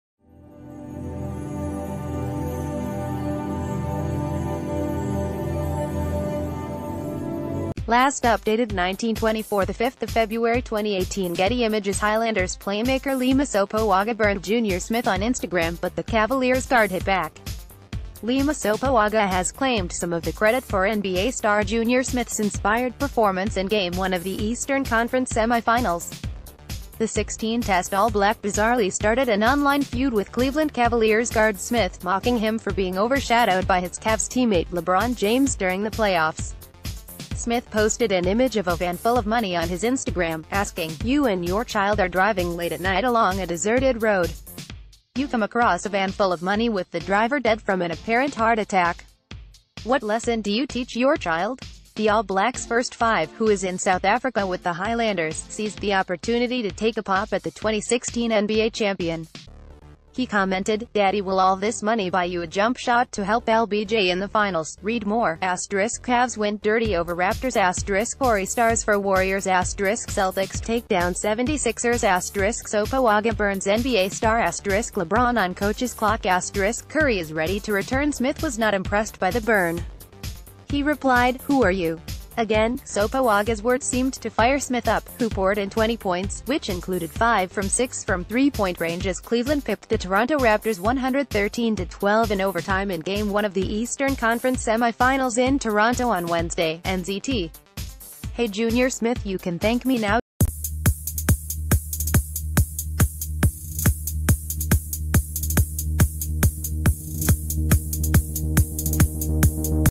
Breaking News | Sopoaga fires up NBA star
Last updated 19:24 02/05/2018 GETTY IMAGES Highlanders playmaker Lima Sopoaga burned JR Smith on Instagram but the Cavaliers guard hit back. Lima Sopoaga has claimed some of the credit for NBA star JR Smith's inspired performance in game one of the Eastern Conference semifinals. The 16-test All Black bizarrely started an online feud with Cleveland Cavaliers guard Smith, mocking him for being overshadowed by his Cavs' team-mate LeBron James during the playoffs. Smith posted an image of a van full...